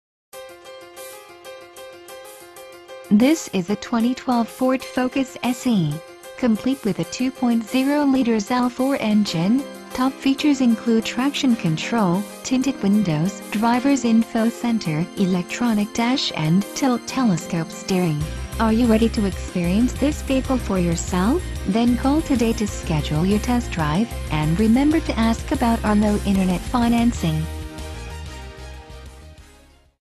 2012 Ford Focus in Apache Junction AZ
At Robert Horne Ford , our long-standing customers know us a dependable source of new vehicles, like this Ford Focus SE4 Door Hatchback , conveniently located in Apache Junction, Arizona . For the absolute best in service, selection, and value, please visit our to learn more about this exceptional 2012 Ford Focus

2012 Ford 4 Door Hatchback SE4 Door Hatchback

Engine: L4, 2.0L; FFV
Fuel: Gasoline
Transmission: Automatic
Exterior Color: White
 Stock #: 12142
 VIN: 1FAHP3K21CL262858